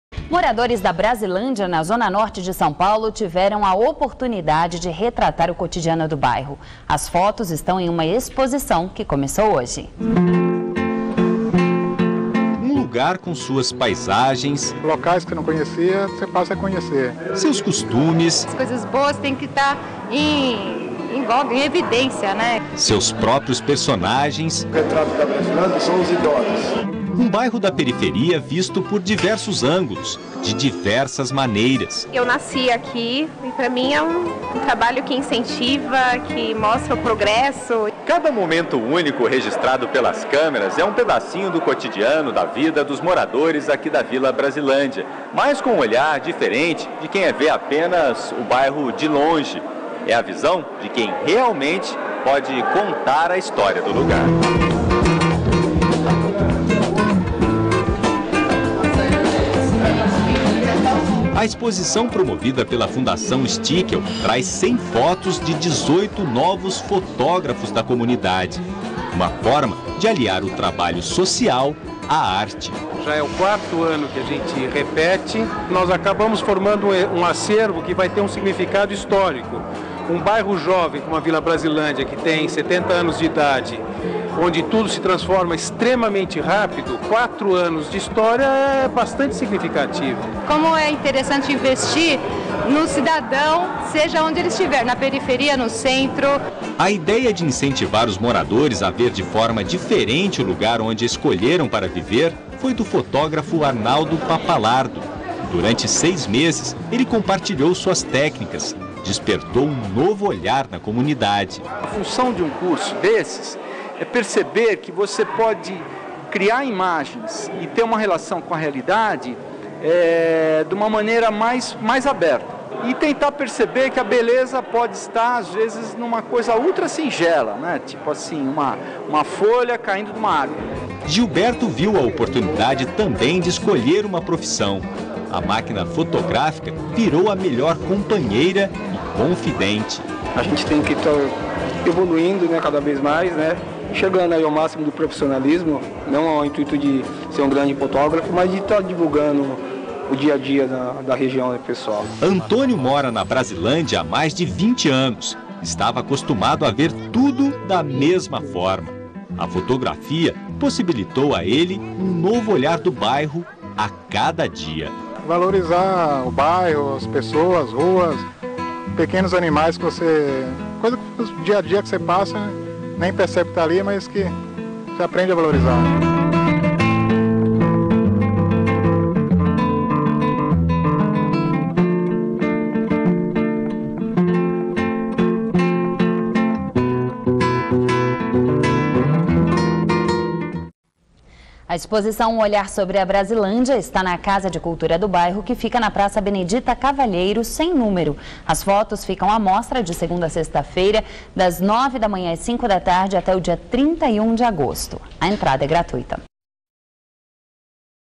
Exposição fotográfica feita por moradores da Brasilândia
Moradores da Brasilândia na zona norte de São Paulo tiveram a oportunidade de retratar o cotidiano do bairro. As fotos estão em uma exposição que começou hoje

Repórter: Vinícius Marra

Tema: Exposição